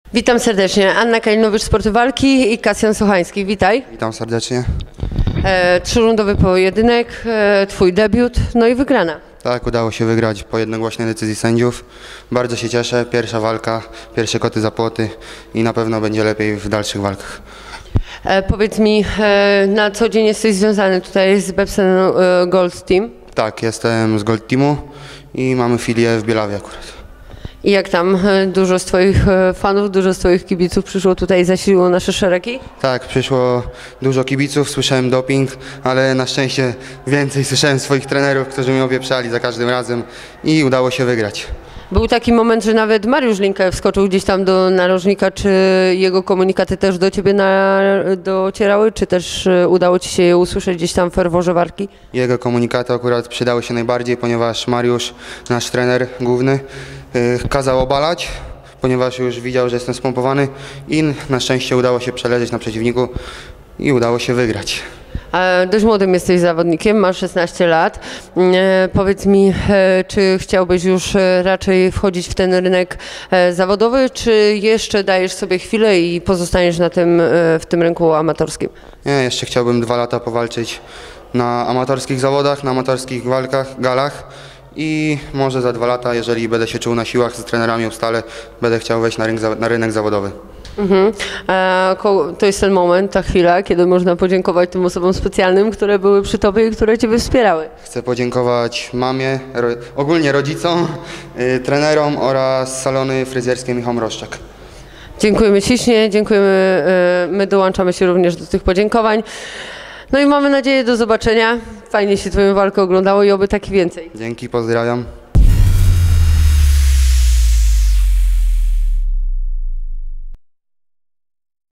Kasjan Sochański po Bebson Arena 3 w Bielawie
- Portal o wszystkich sportach walki przedstawia wywiad zarejestrowany po gali Bebson Arena 3 w Bielawie.